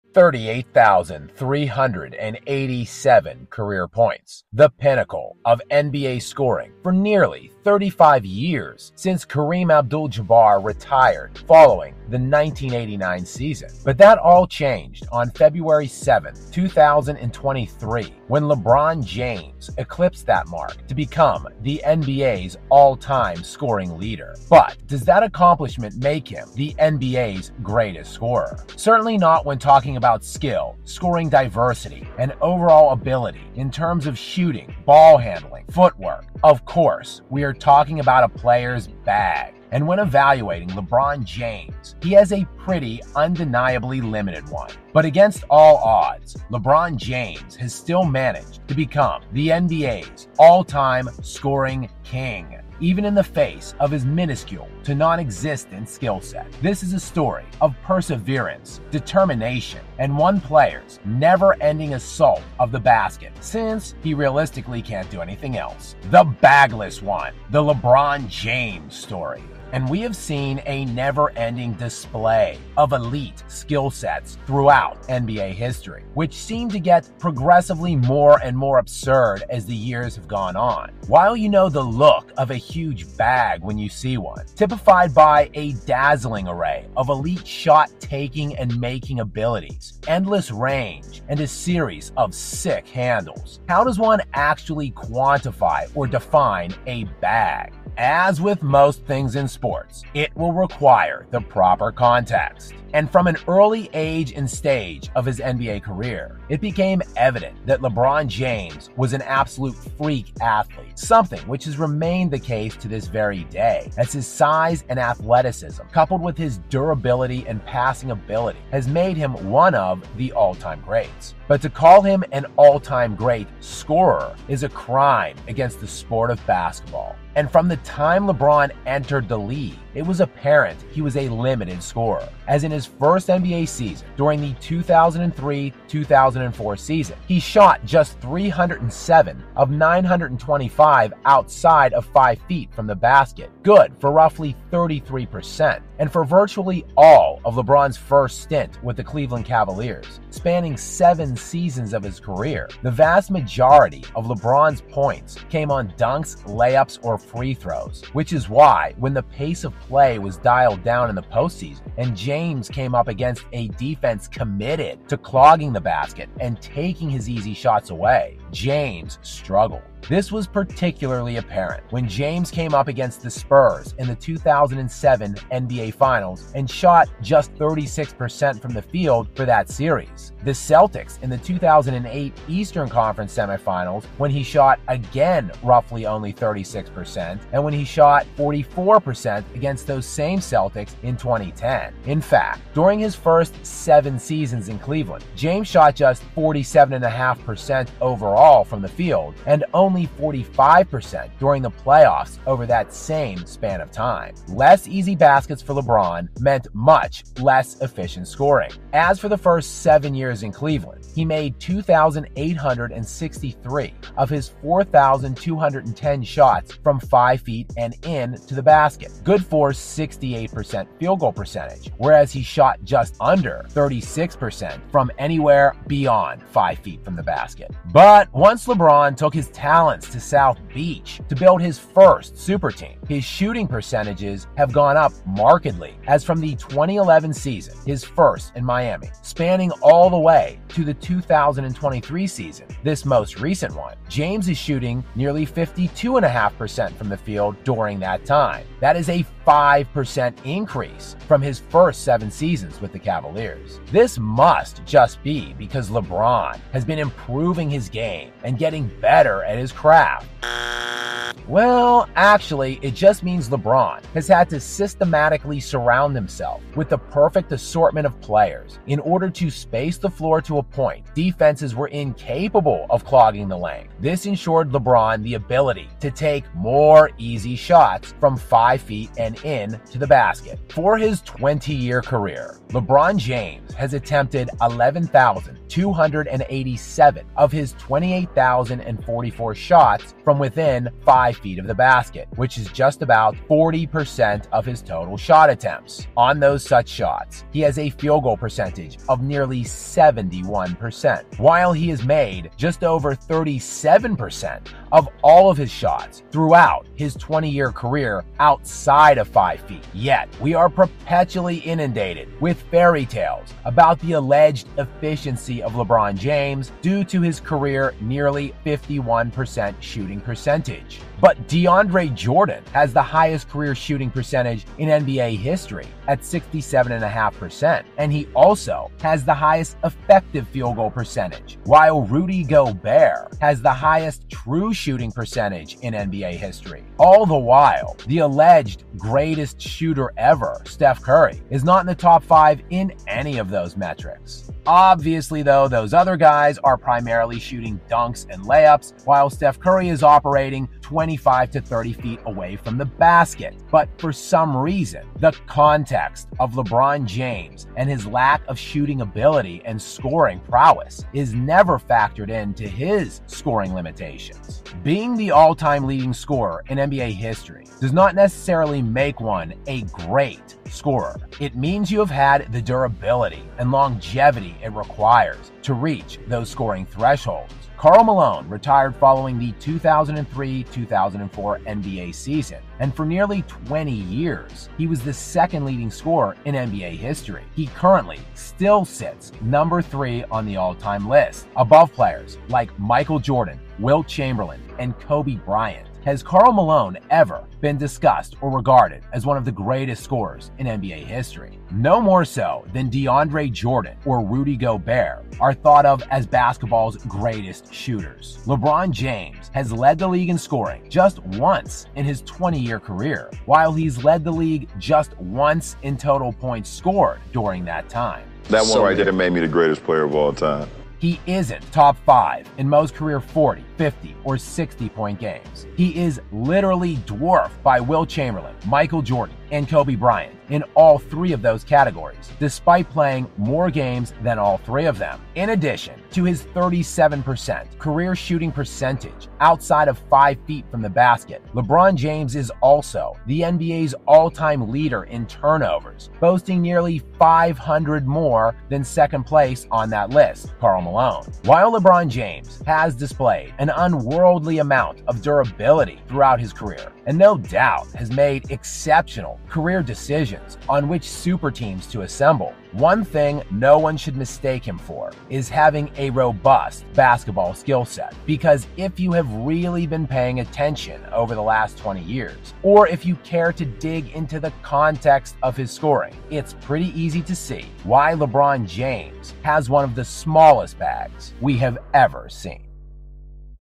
The Bagless One: The LeBron James Story
LeBron James has NO bag. Here is the definitive proof.